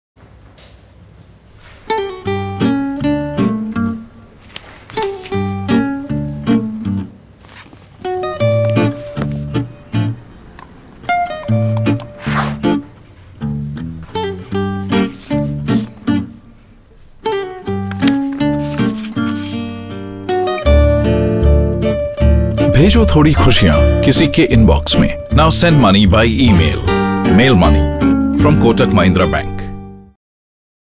Kotak Super Banking Mail Money Book Store
Kotak Super Banking - Bhejo thodi khushiya kisi ke inbpx mein, now mail money through email.

Agency - Cartwheel Creative
Brand - Kotak Mahindra Bank
Production House - Jamic Films
Director - Nikhil Rao